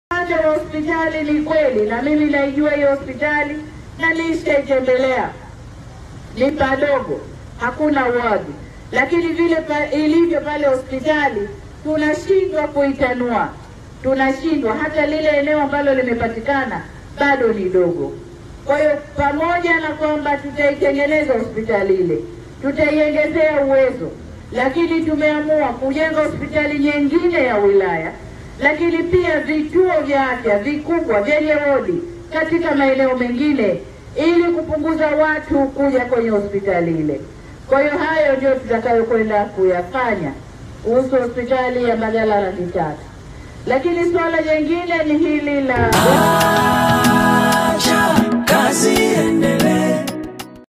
Ufafanuzi Kuhusu Hospital ya Mbagala na Kuahidi Kufanya Maboresha na Kujenga Vituo vya Afya zaidi.
Rais wa Tanzania na Mwenyekiti wa CCM Ndugu Samia Suluhu Hassan ametolea Ufafanuzi Kuhusu Hospital ya Mbagala na Kuahidi Kufanya Maboresha na Kujenga Vituo vya Afya Zaidi Kwenye Maeneo Mengine.